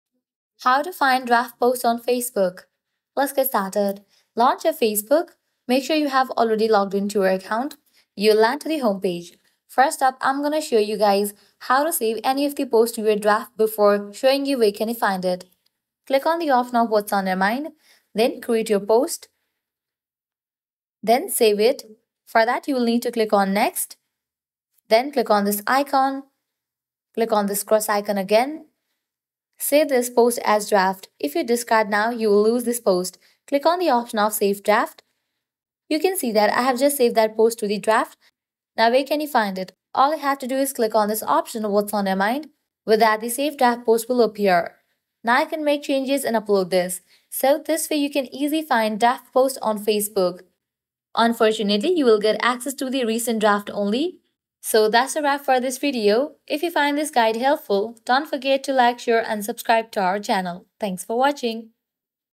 How To Find Draft Post On Facebook - EASY GUIDE
Looking for your draft post on Facebook? This guide will show you how to easily find and manage your draft posts on Facebook in 2024.

Timestamps:
0:00 - How to Find Draft Posts on Facebook
0:10 - Steps to Access and Edit Drafts

FAQs:
- Where can I find my saved drafts on Facebook?
- Can I edit or delete a draft post before publishing?
- What should I do if my draft post is missing?
- Are there any restrictions on saving drafts on Facebook?